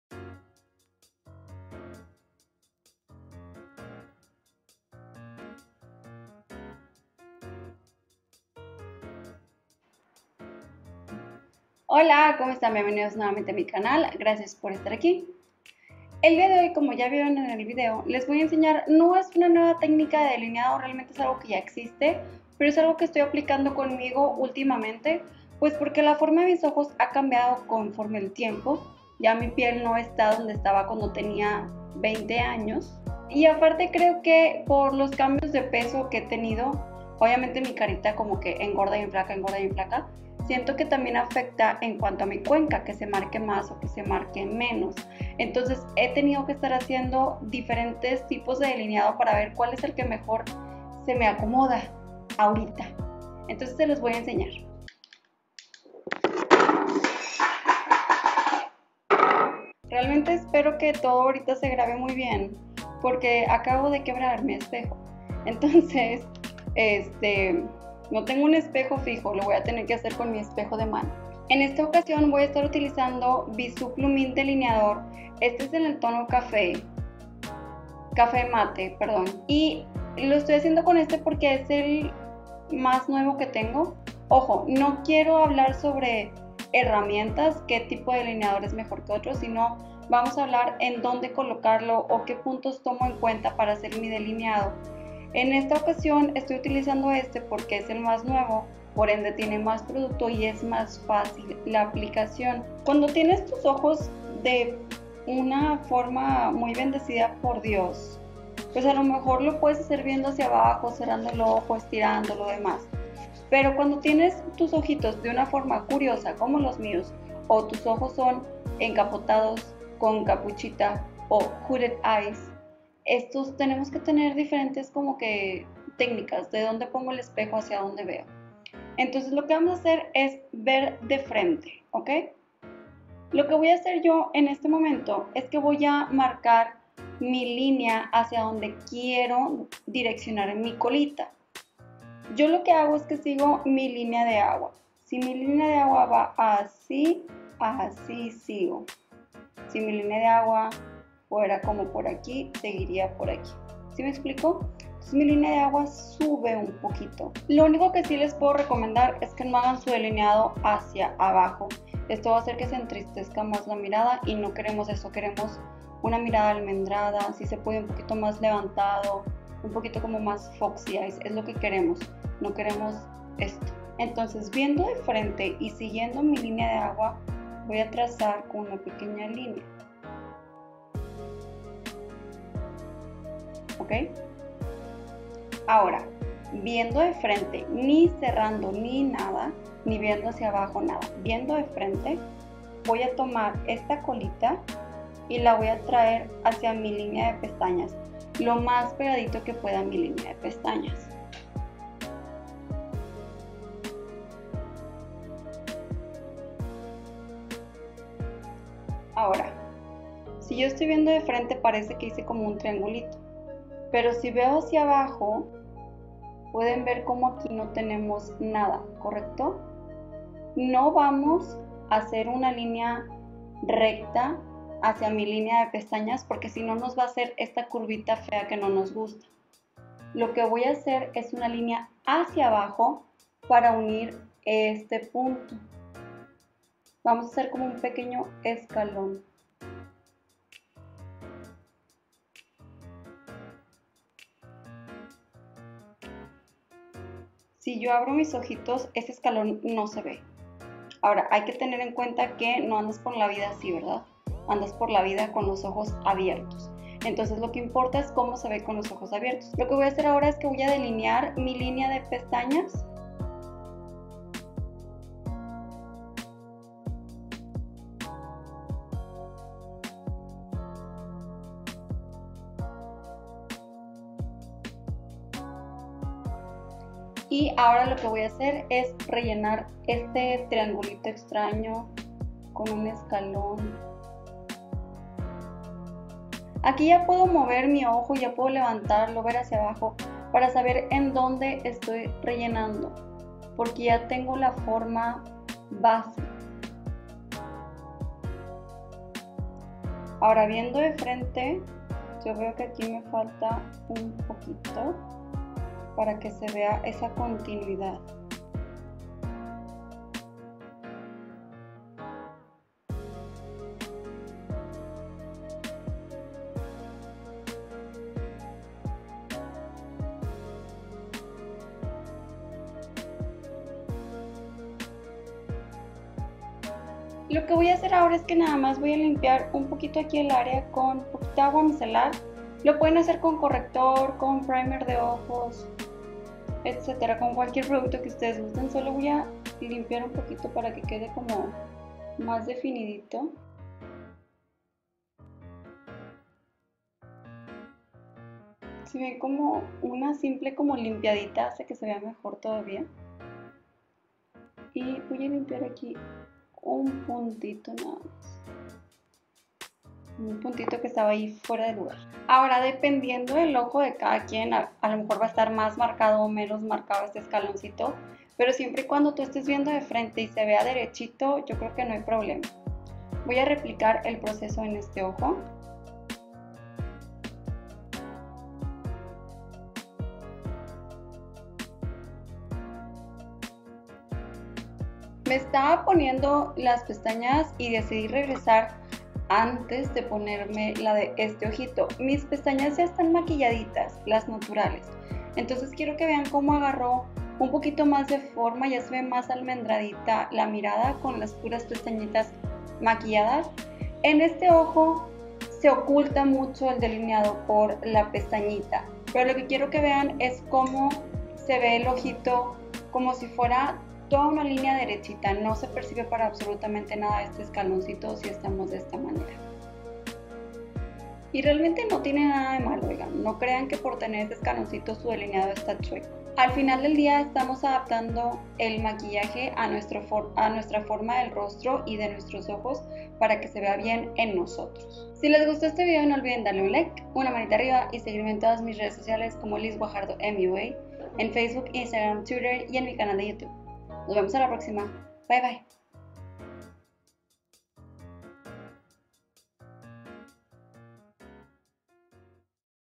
Delineado escalonado | Delineado para párpado semi caído | Liz Guajardo MUA
Pues como dice el titulo del video, hoy veremos algo muy básico y sencillo que es un delineado, pero si tienes los ojos problemáticos como los míos, no te puedes perder este tutorial.

Si quieres brincarte toda la explicación y solo ver como me delineo los ojos, brinca hasta el minuto 3, pero igual la explicación también te puede ayudar hahaha. Espero les sirva este video, enjoy!